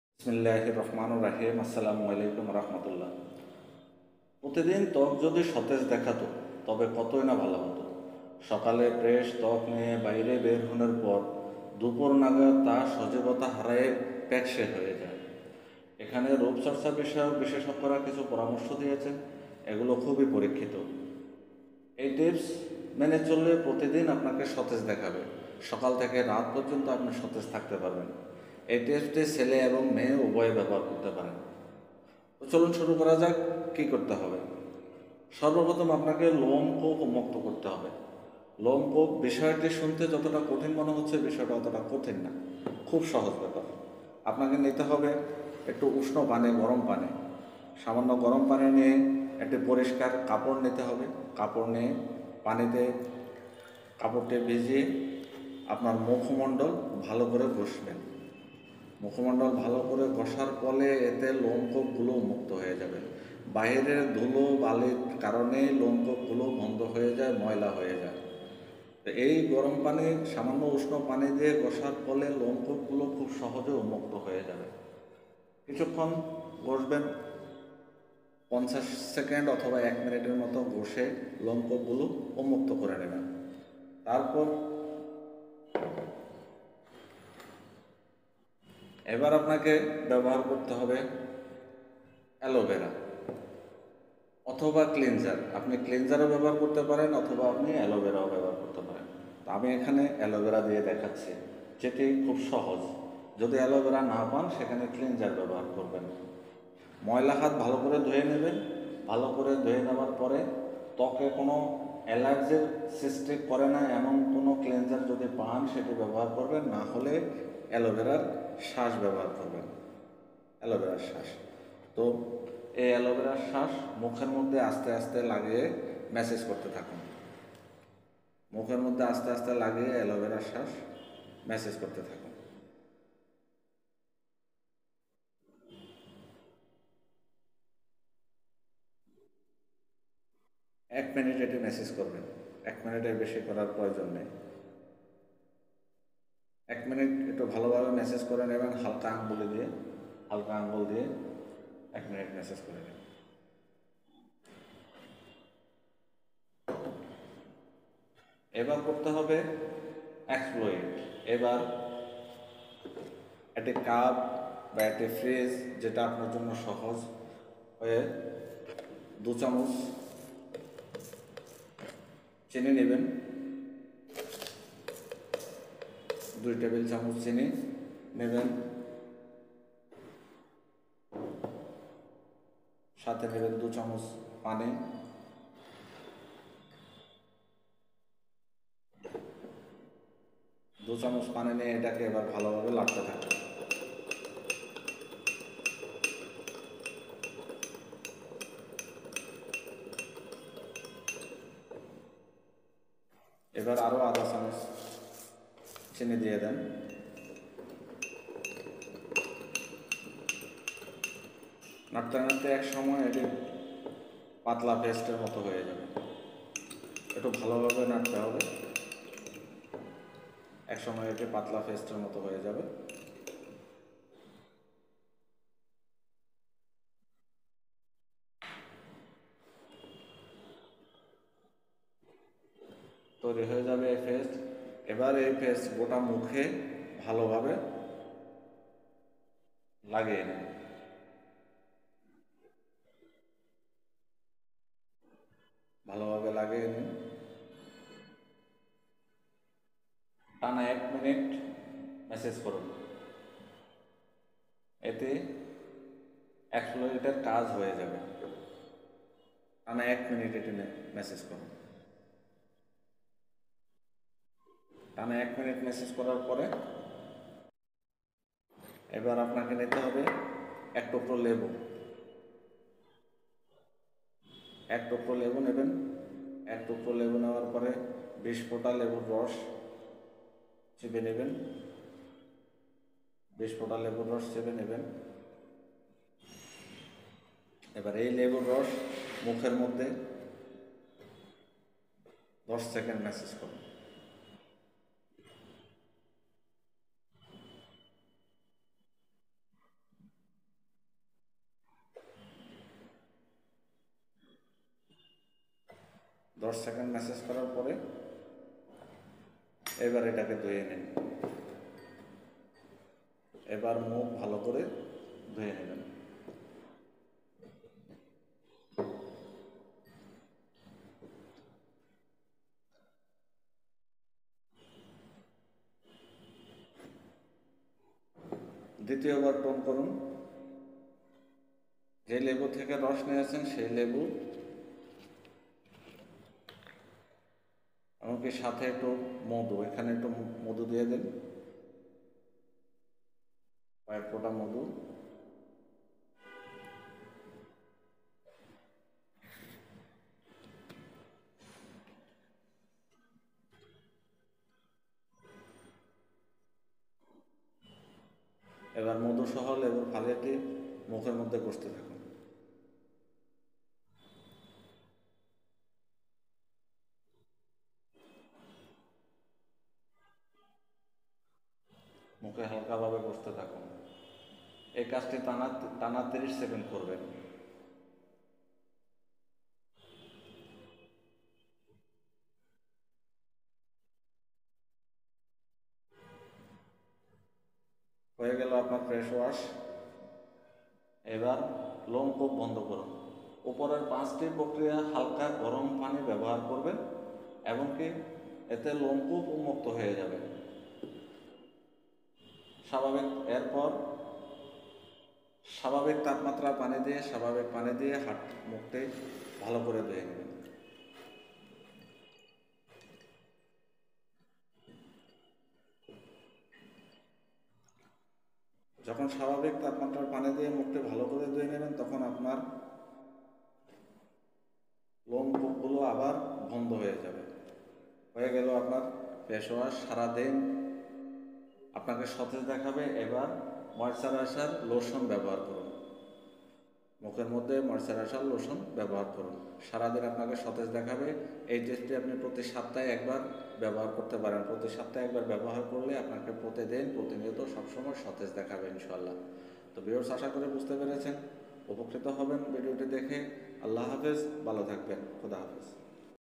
Haw to get fresh skin every day |Effective tips to get fresh skin every day | bangla health tips
Haw to get fresh skin every day |Effective tips to get fresh skin every day |

If the skin looked fresh every day. But how good it would be. After going out in the morning with fresh skin, it loses its vitality by noon. The experts in the field of appearance here have given some advice. These are tested. If you follow these tips, you will look fresh every day.

Effective tips to get fresh skin every day
প্রতি দিন ত্ব যদি সতেজ দেখতো। তবে কতইনা ভালো হতো।সকালে ফ্রেশ ত্বক নিয়ে বাইরে বেরুনের পর দুপর নাগাদ তা সজিবতা হারায়।এ খানে রুপ চর্চা বিষয়ক বিশেষজ্ঞরা  কিচু পরামর্শ দিয়েছেন। এ গুলো পরিক্ষিত।এ টিপস মানলে প্রতি দিন সতেজ দেখাবে আপনাকে।।।

subscrib this channal.